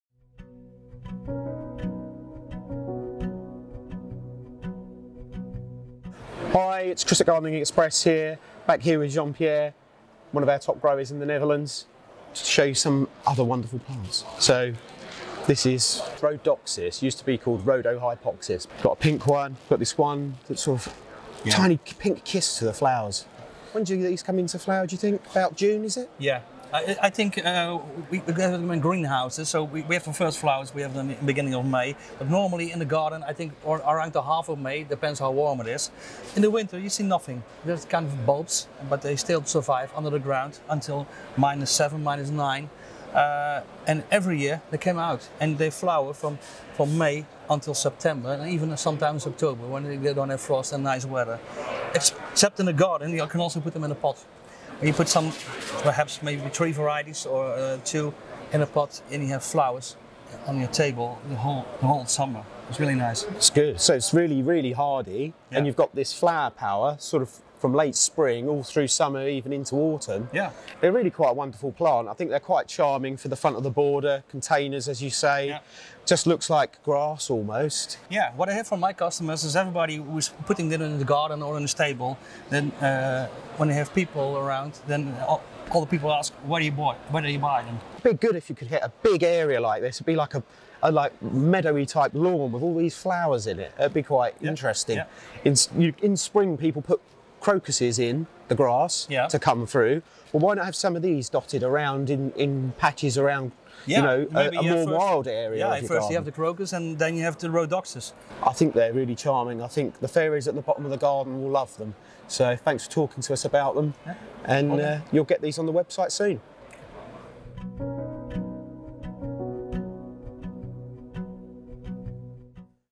Rhodohypoxis Selection - Beautiful Rhodoxis Star Grass
Filmed at the Plantarium|Groen-Direkt fair in Holland, managing director Chris speaks to talented plant grower Pierre about Rhodoxis. This particular plant is a very choice specimen for an alpine garden, rockery or trough planting, producing masses of Twinkling Star-shaped flowers.

Plants featured in this video Rhodohypoxis Twinkle Stars - Fairytale Rhodoxis - Magenta Star Grass Rhodohypoxis Twinkle Stars

Rhodohypoxis is a bulbous South African native with grassy leaves and star-shaped blooms from May to August on plants reaching just 10-15 cm tall. It is supplied in approx 2 litre containers. Full of buds and flowers in season. They can be grown in patio containers, an old sink, between paving stones, in dry-stone walls or a rockery - so they are perfect for small, modern gardens, patios or balconies or simply at the front of a garden border.